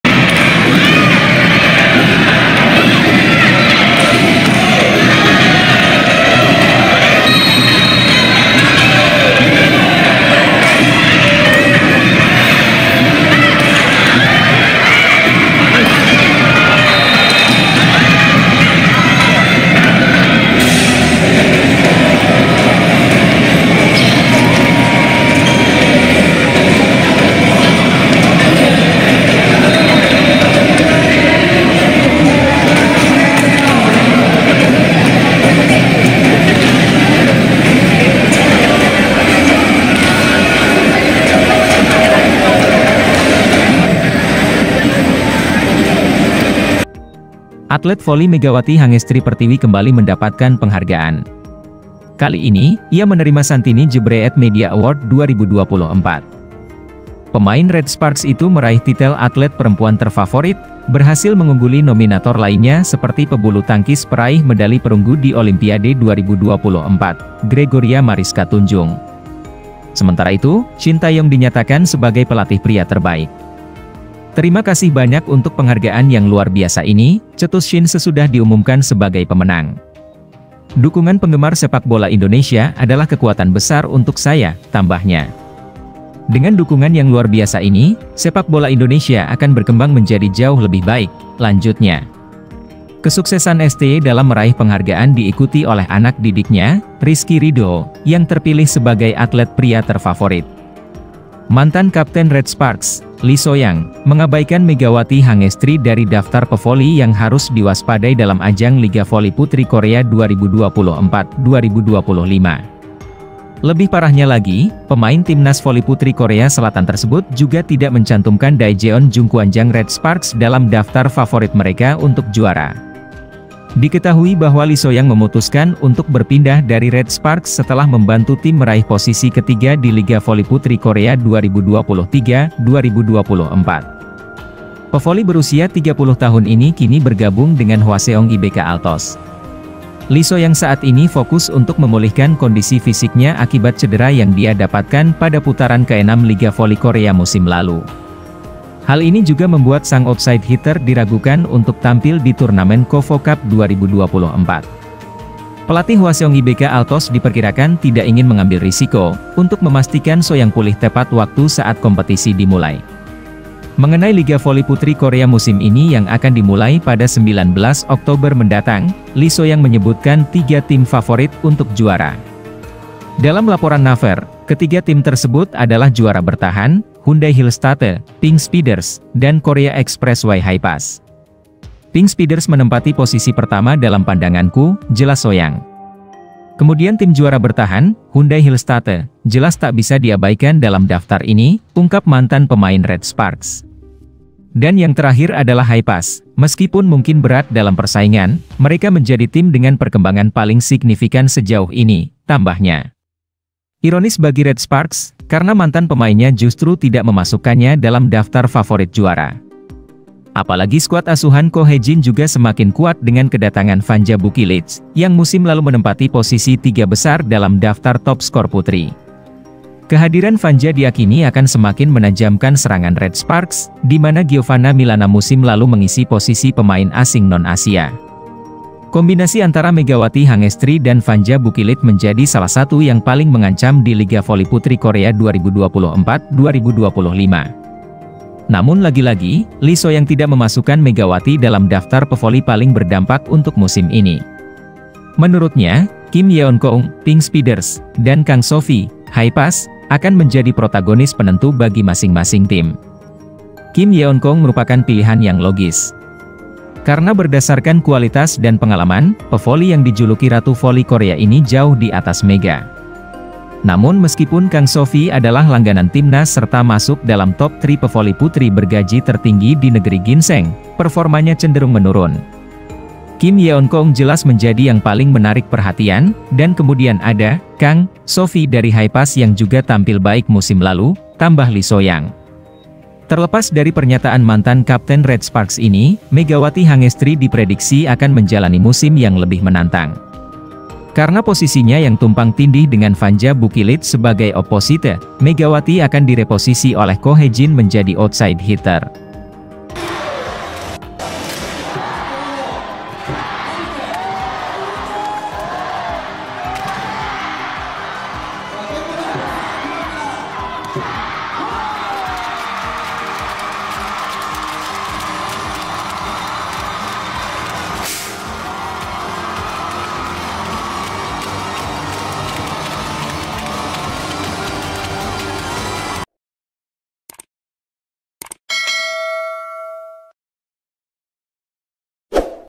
KO HEE JIN BERI LATIHAN KHUSUS UNTUK MEGAWATI AGAR LINI SERANG RED SPARKS SEMAKIN KUAT & MEMATIKAN!
BERITA OLAHRAGA DUNIA UNTUK INDONESIA!
Megawati Hangestri Liga Korea
Jadwal Pertandingan Megawati Liga Volley Korea & Proliga 2024